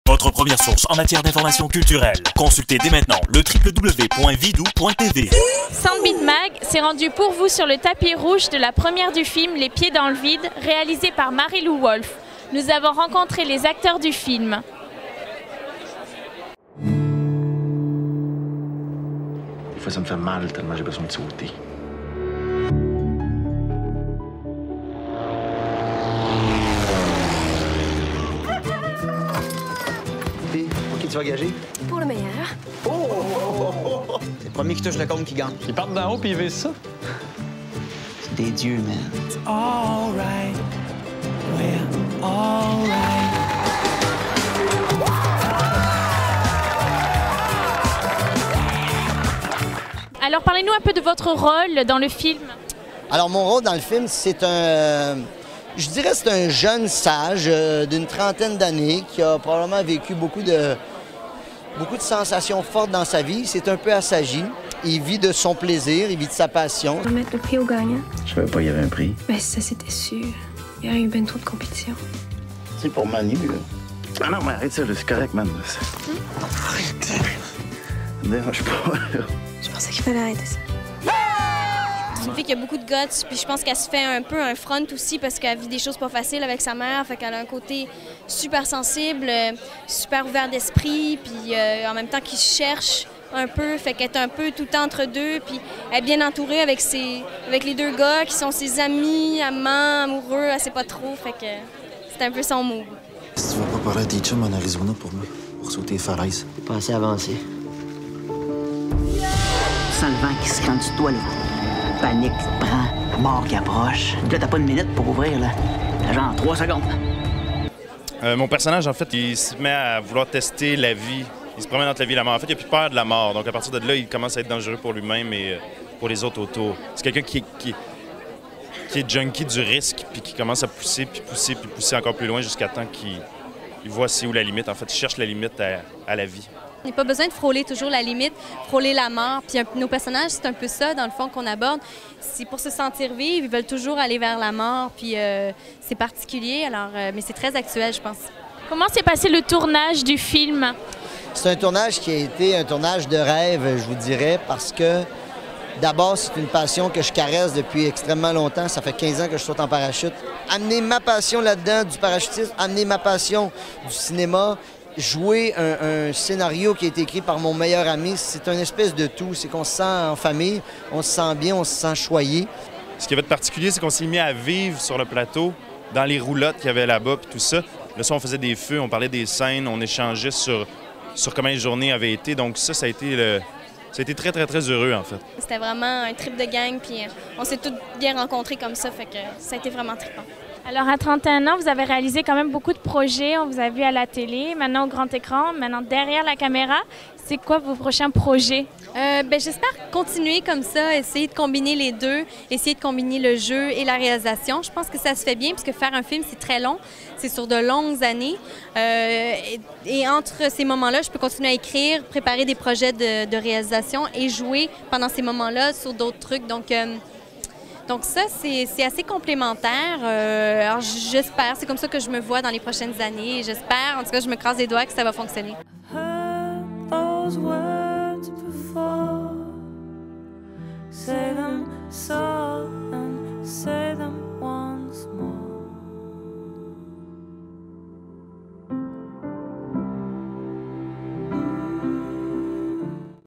Les Pieds dans le vide
SoundbeatMag était sur le tapis rouge du premier long métrage de la charmante Mariloup Wolfe, Les Pieds dans le vide, afin de discuter avec elle et quelques comédiens du film. Entrevues et montage par Aude Constantin.